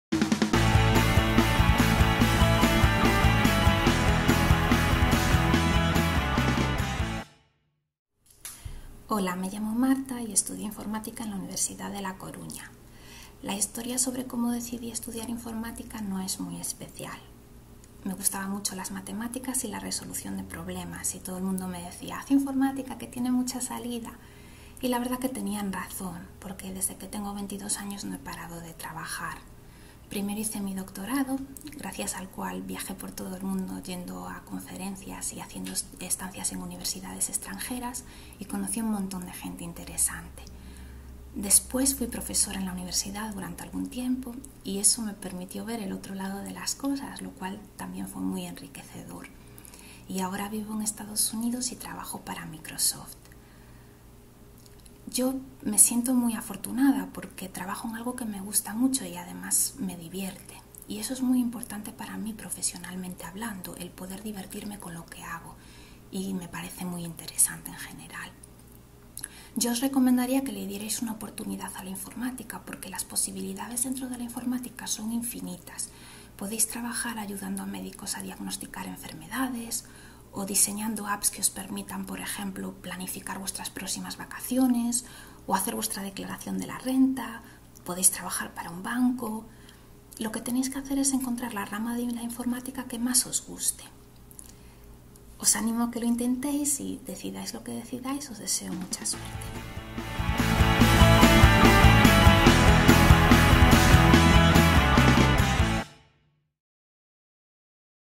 ICT-Go-Girls! Marta Penas Centeno, Ingeniera de Microsoft.
ICT-Go-Girls! Martas Penas Centeno fue investigadora y profesora en la UDC. En la actualidad trabaja en Seattle como ingeniera de desarrollo en Microsoft. En este vídeo, anima a las chicas a estudiar TIC y cuenta su experiencia.